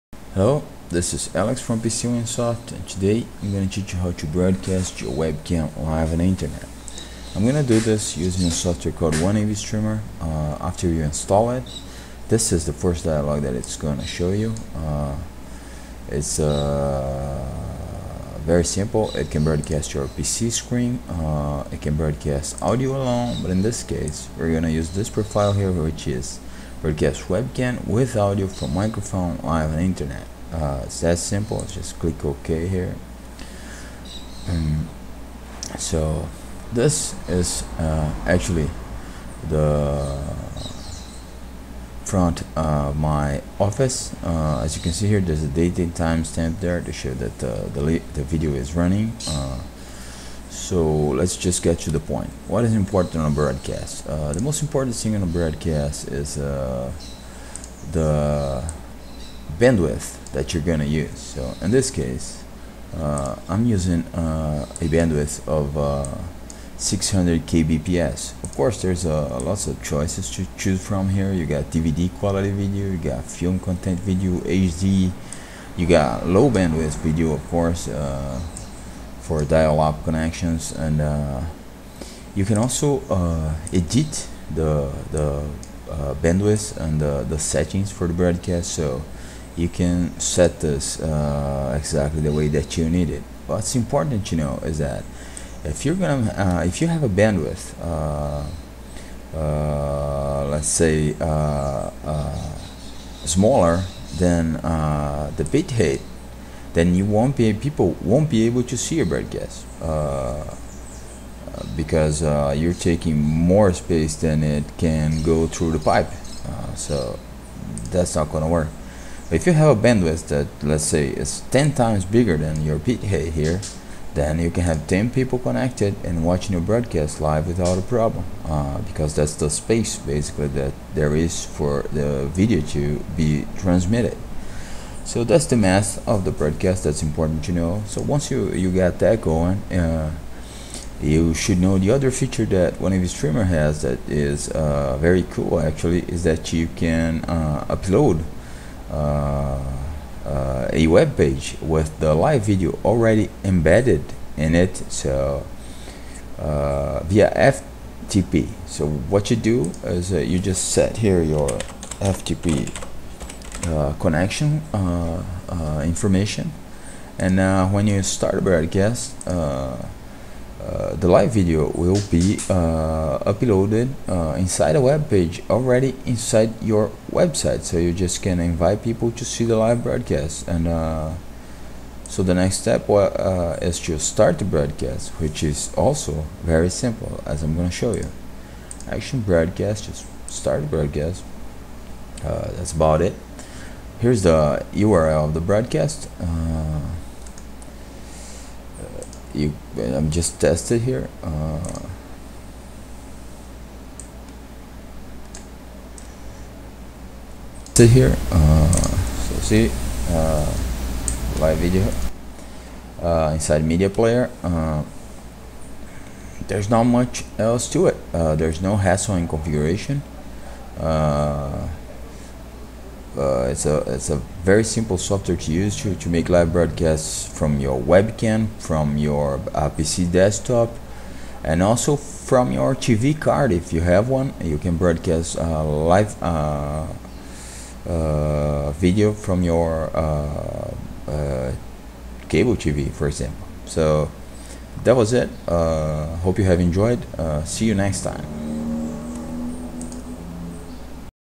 How to stream your webcam live on the Internet
Learn how to stream your webcam live on the Internet. This same tutorial will teach you how to broadcast your desktop live on the Internet, and it will also teach you how to broadcast your TV card signal live on the Internet.